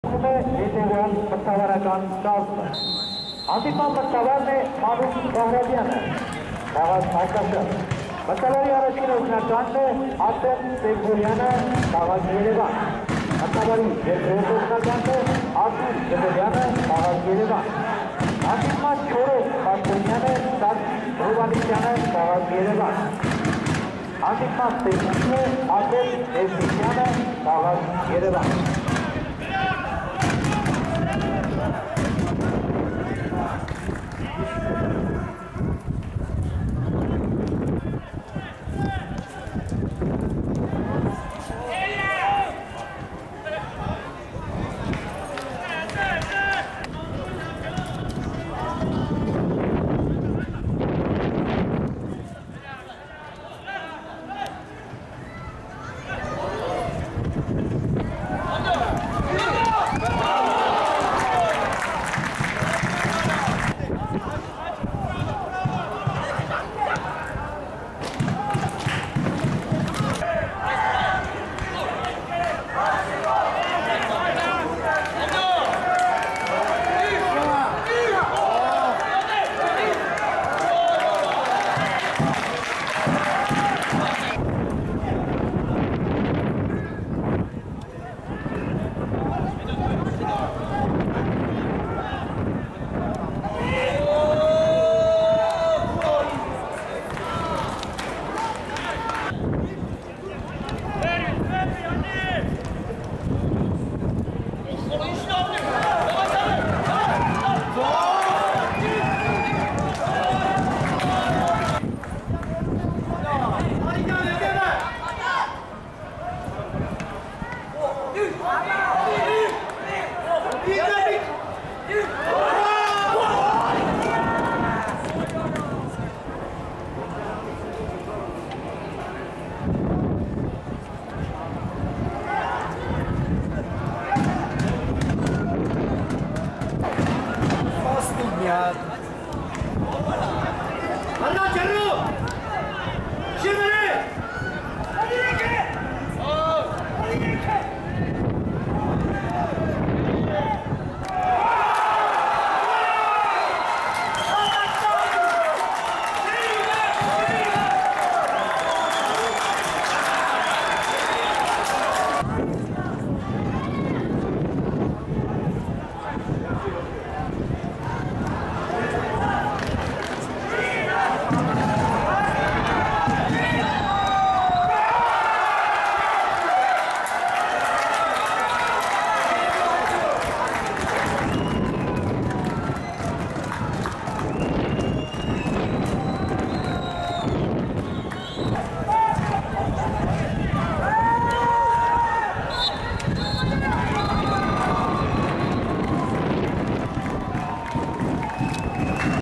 FC Ararat - FC Shirak 0:2, APL, Week 02 (2014/15)
FC Ararat Yerevan - FC Shirak Gyumri 0:2 (0:0)
09 August 2014, Armenian Premier League 2014/15, Week 02.
Goals: Jean-Jacques Bourgoin, 83 (0:1), Jean-Jacques Bourgoin, 90 (0:2)
HQ Extended highlights by Armenian Soccer.
Yerevan, Hrazdan Stadium, Attendance: 250